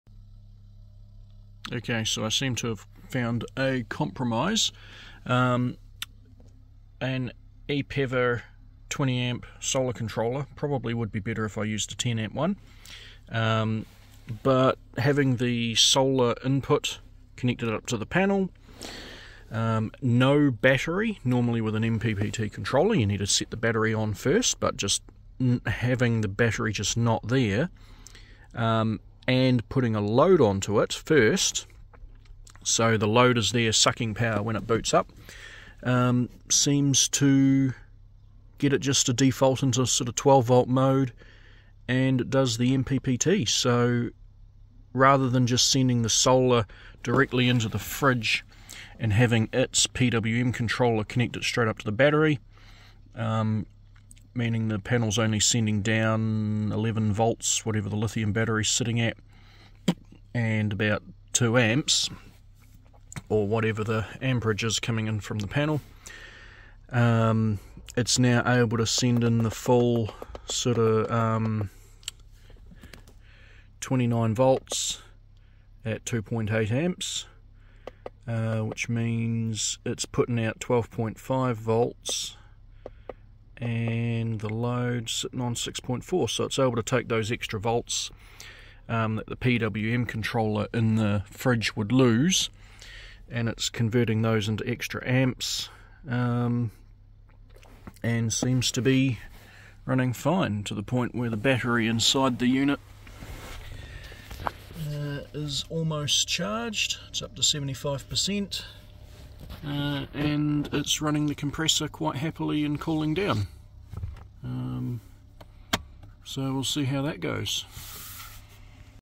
Brass Monkey Fridge MPPT Solar Solution
Brass monkey fridges appear to only have a PWM controller. This means any voltage above 12v that your panel puts out will be lost.
Translation for a cloudy day:
My 180w panel on a slightly overcast day might be able to put out 28v at 2.0 amps.
If the panel is directly connected to the input of the fridge, it will only accept 11v at 2.0 amps so the 180w panel is reduced to 22 watts.
If it runs via an MPPT controller (which the fridge really should have included) then it will do 28v at 2.0 amps and through some conversion be operating at 56 watts. Much better.

On a sunny day the PWM reduces the panel down to 56 watts absolute maximum where as if it had an MPPT controller, the fridge could accept close to 120 watts.

We oversize panels as a general rule so that we offset less efficiency on a cloudy day and still capture enough to run the fridge.

According to the instruction manual, the fridge uses an average of 0.2kwh per day or 33watt hours per hour. At 12v that means the fridge needs a 2.7 amp supply for 5 hours per day. Thats not possible with most panels on a cloudy day unless you do some MPPT conversion to increase their performance.